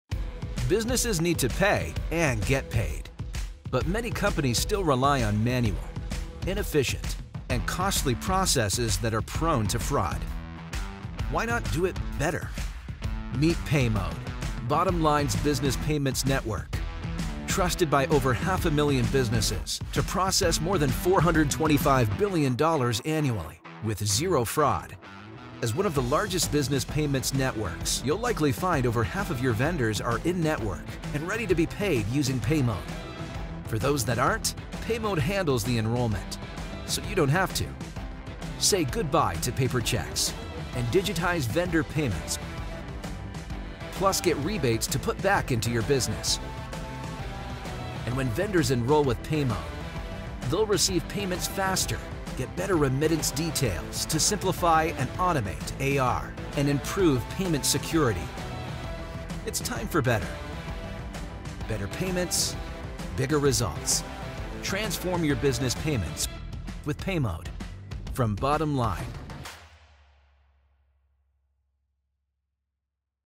Paymode Business Payments Network | Secure, Efficient AP Automation
See how Paymode can drive efficiency and cost savings for your business

Paymode, Bottomline’s business payments network, securely processes over $425B annually with zero fraud. Over 550,000 verified and authenticated businesses are on the network today, paying vendors securely and earning rebates from AP spend.

Paymode has long been recognized as a leading business payments network, bringing benefits to both payers and vendors.